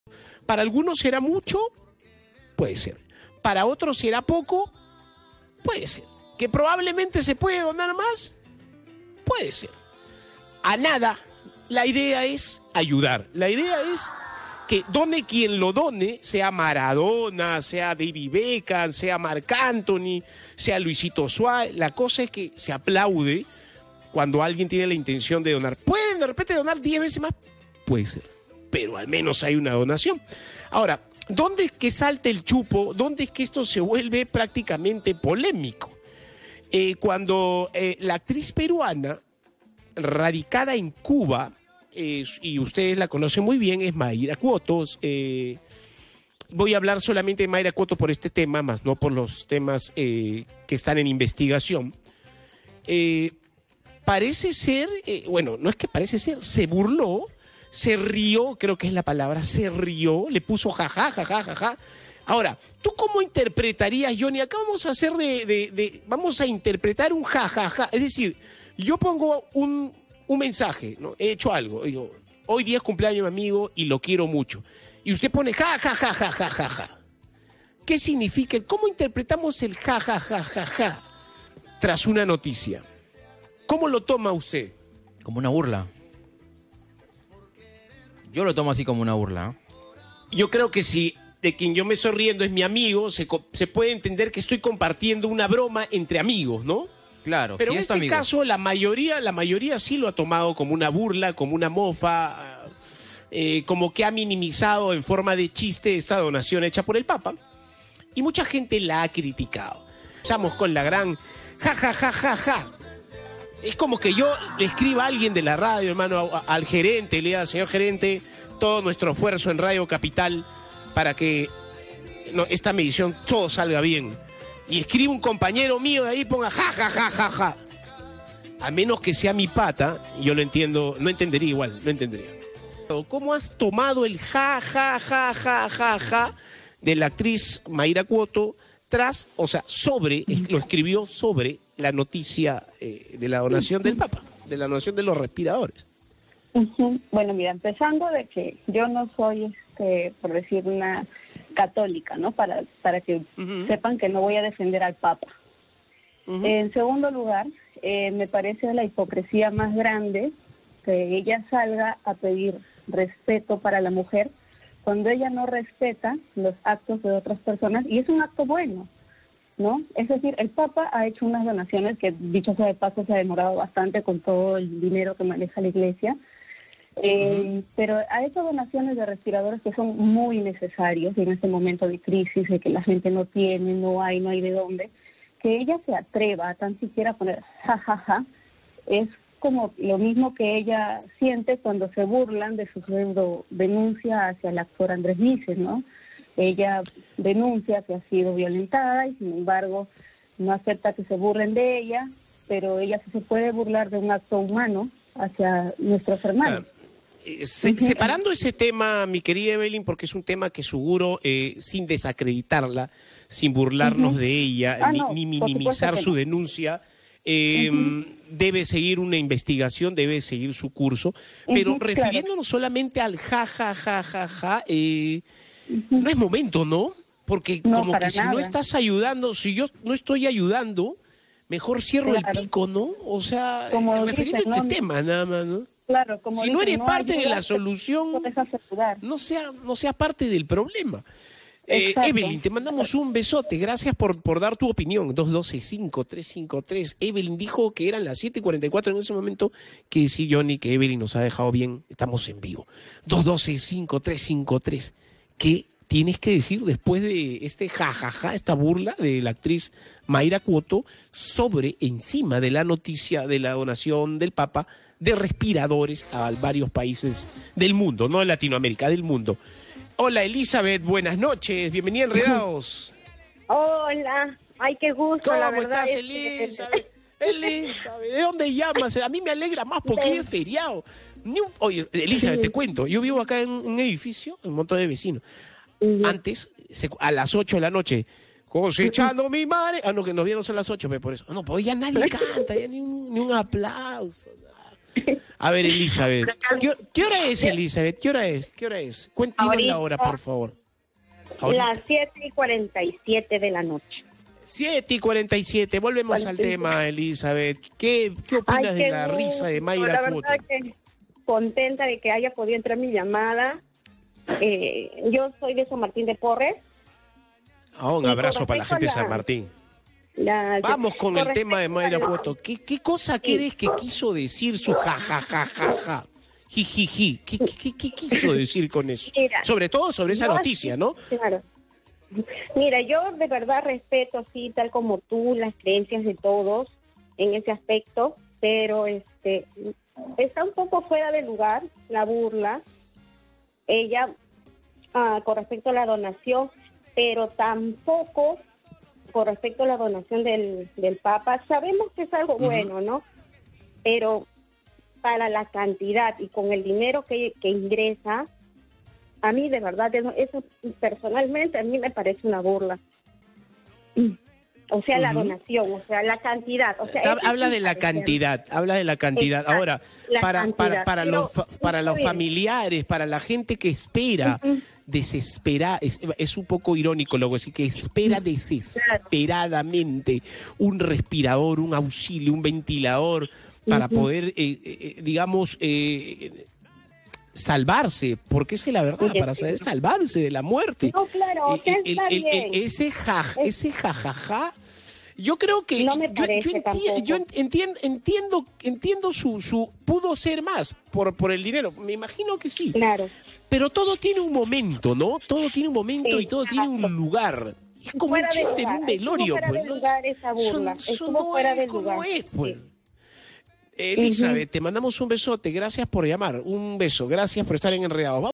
Adolfo Bolívar sobre Mayra Couto: “Si no estoy ayudando, mejor cierro el pico”
“Ese ‘jajaja’ es como un chiste en un velorio”, dijo el conductor sobre la actriz que se burló de la donación de respiradores del Papa Francisco.

No dejes de comentar. En Capital, tu opinión importa